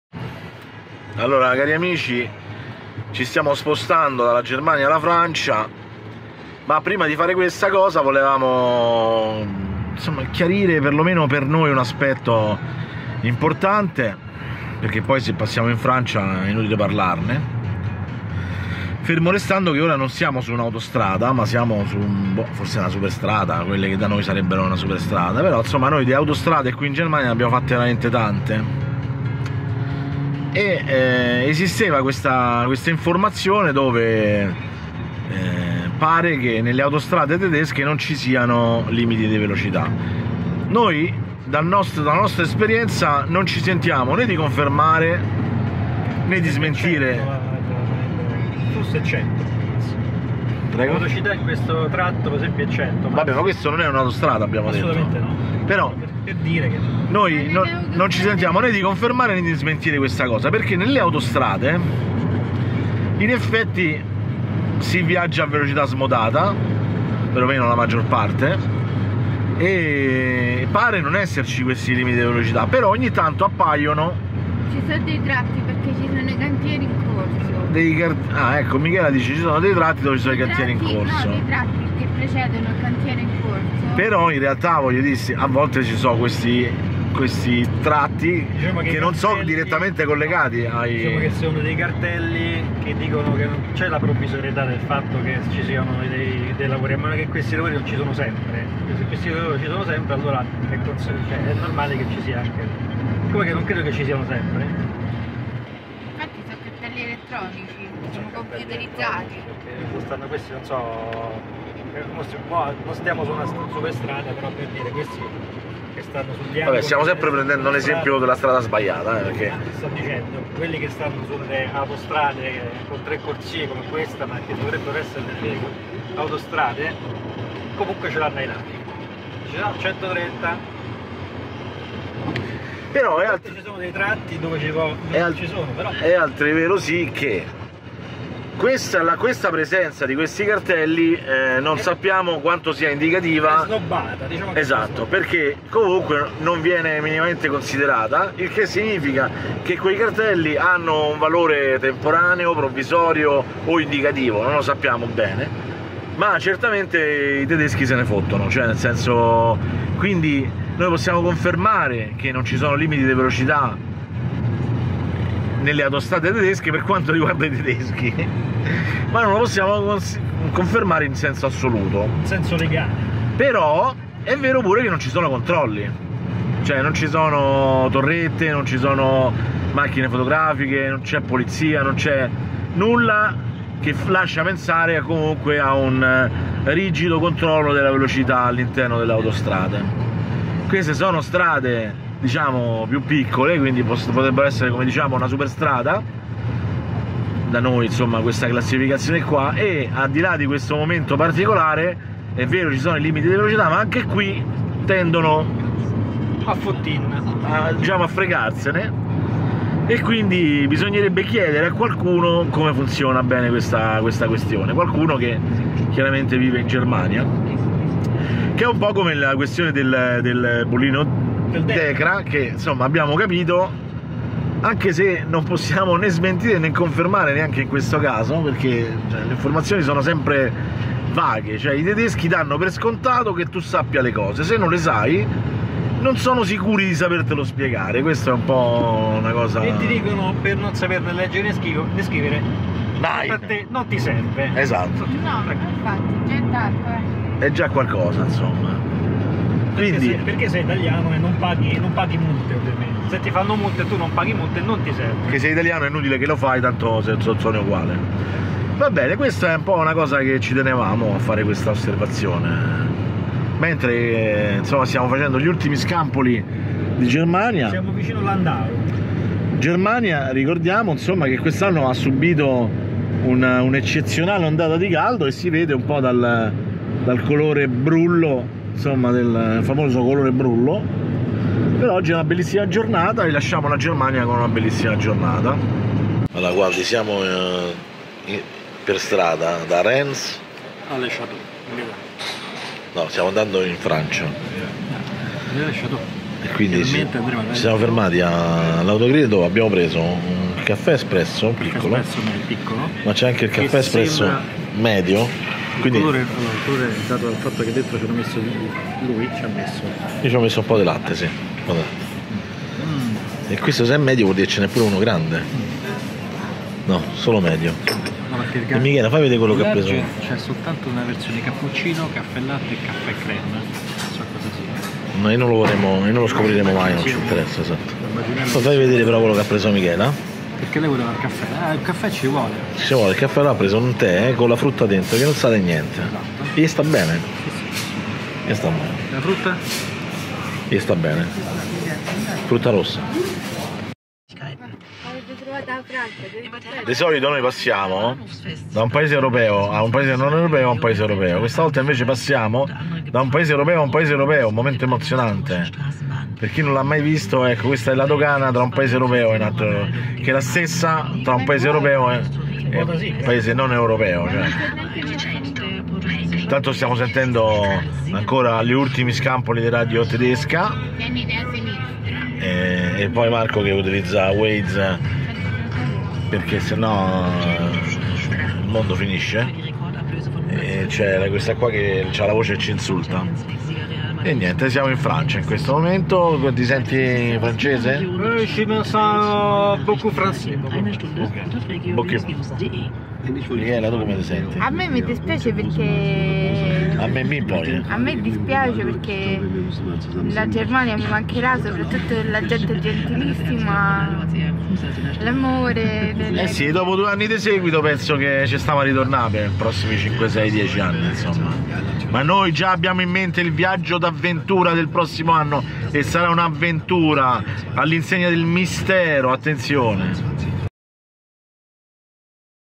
LA GERMANIA E I LIMITI DI VELOCITA' - Renania2018 Travel Log 16
Narra la leggenda che in un paese evoluto e ricco di senso civico, ossia la Germania, non ci siano LimitiDiVelocità nelle Autostrade. Ebbene sarà vero? Diciamo la nostra mentre lasciamo il paese dando fine a Renania2018.